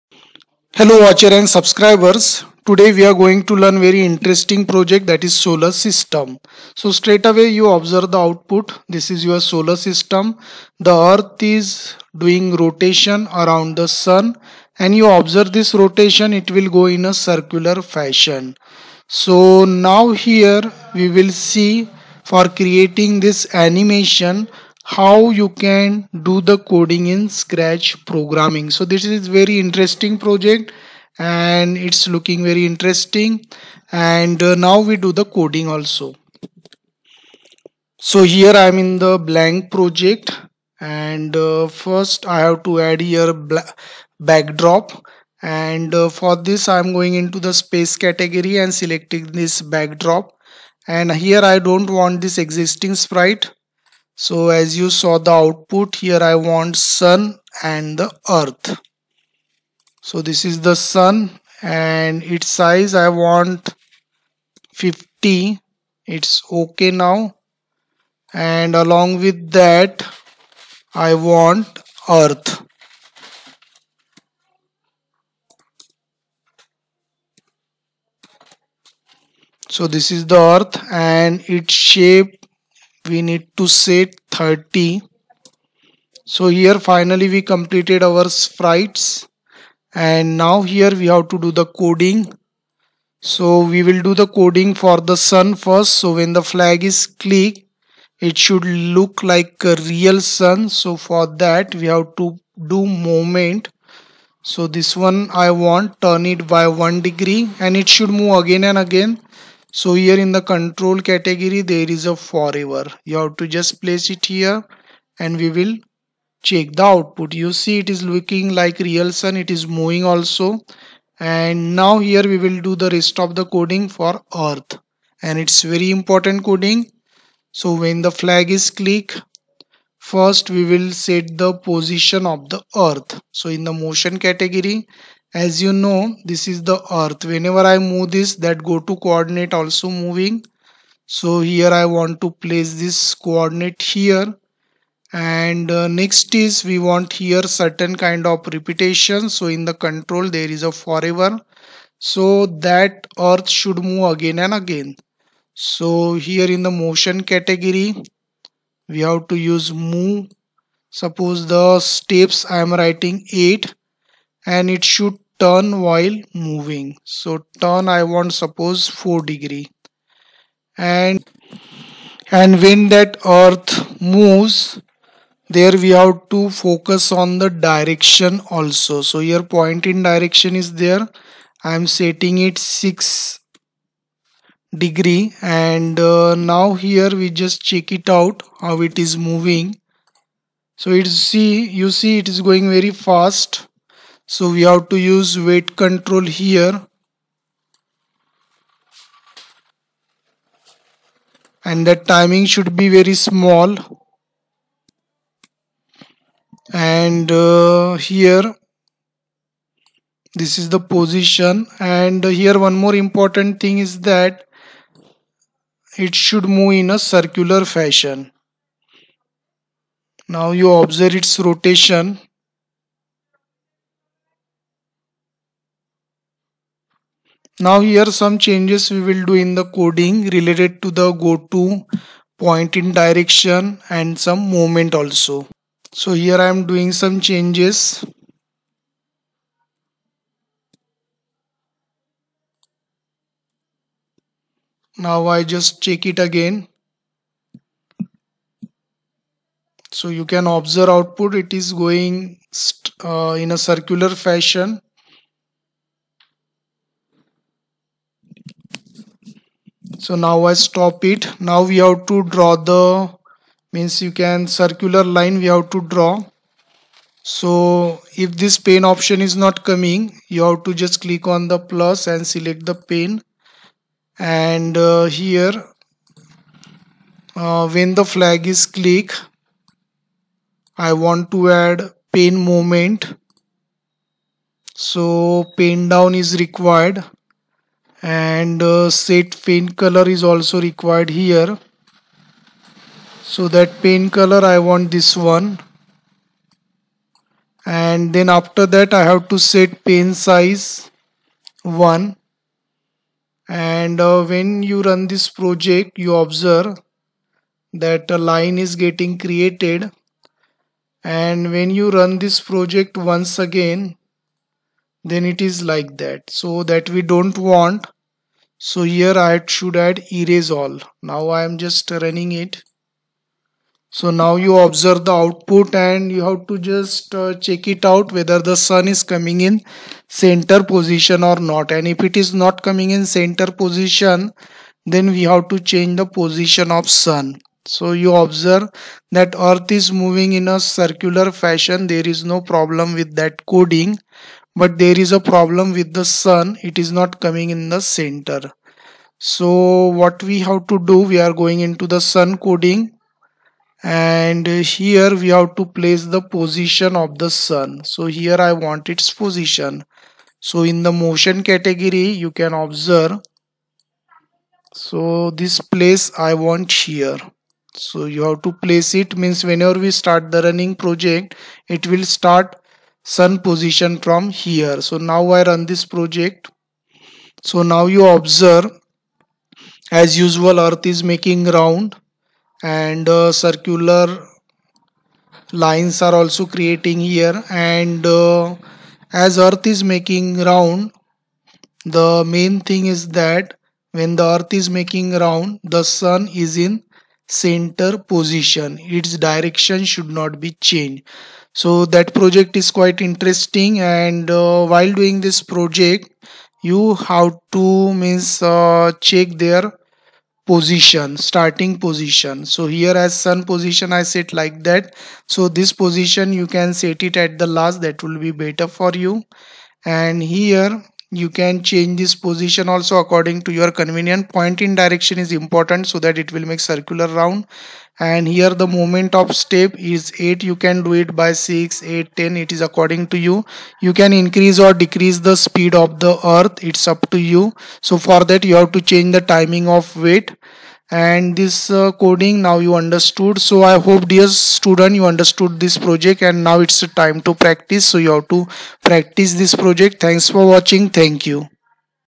31. Solar system project in Scratch
Hello friends,
We will see how to develop  solar simulator in scratch

Learn technical and programming tutorial form IT SKILLS WITH SURAJ DATIR.